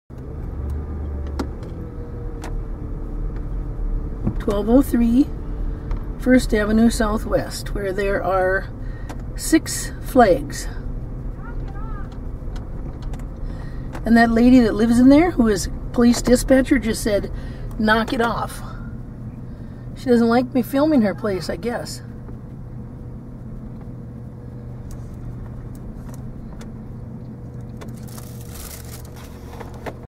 1203 First Ave SW Little Falls Mn Kathy Barthels LFPD dispatcher
12-5-14 notice a woman yells "knock it off" at me. She is upset I am videoing her home.  It is legal to do this. This property at 1203 Southwest First Ave. in Little Falls Minnesota, is clearly in violation of the sign ordinance.   2 flags max per residential property.  It is the law Kathy.